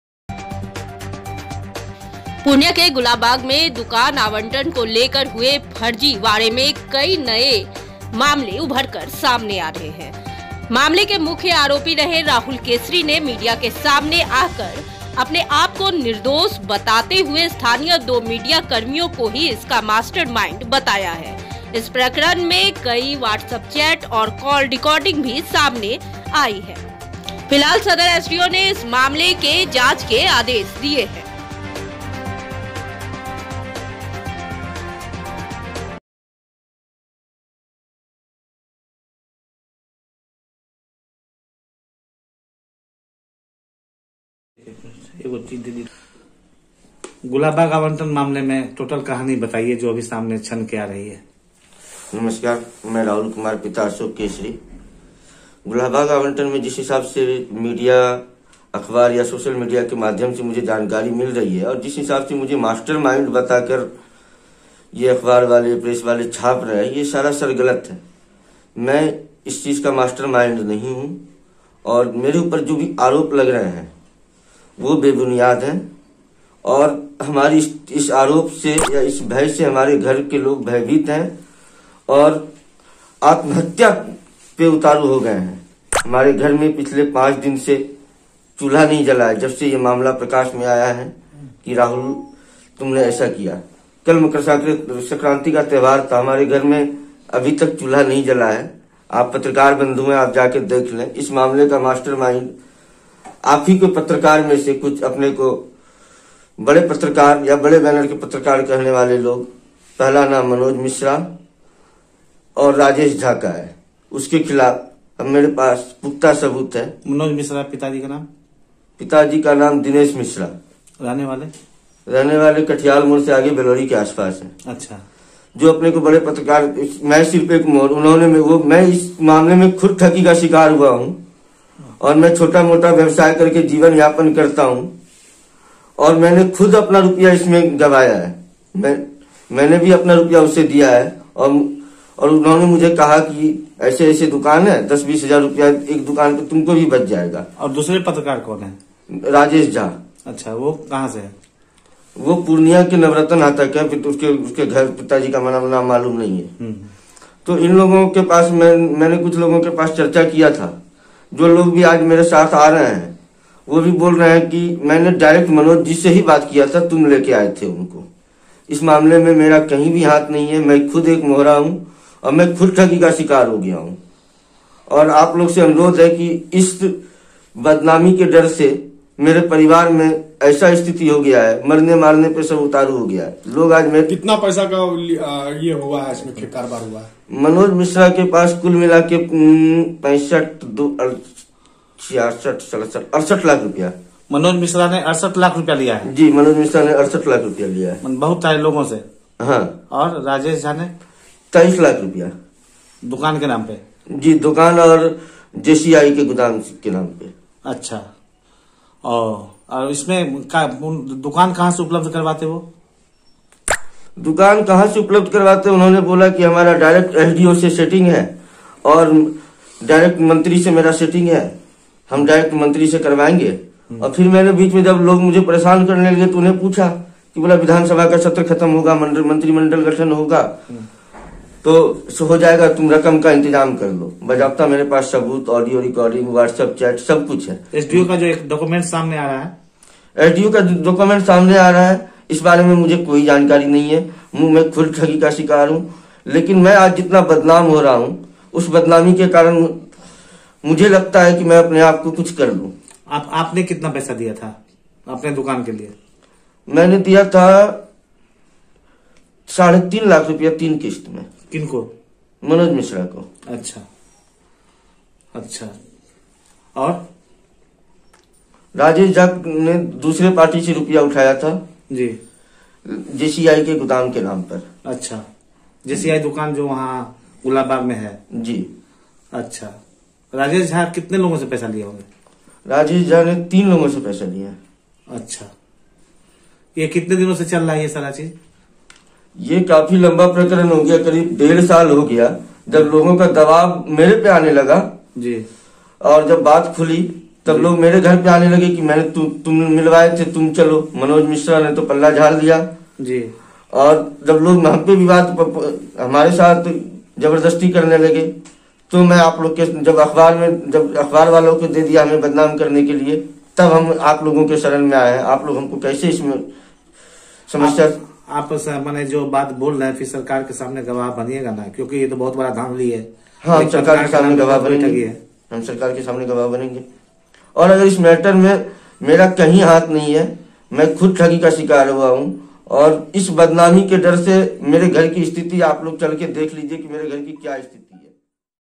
पूर्णिया गुलाबबाग में दुकान आवंटन घोटाला ,कई चेहरे हुए बेनकाब! देखिये रिपोर्ट!
Purnea Gulabbag Bihar सदर एसडीओ ने दिए मामले के ख़िलाफ़ जांच के आदेश।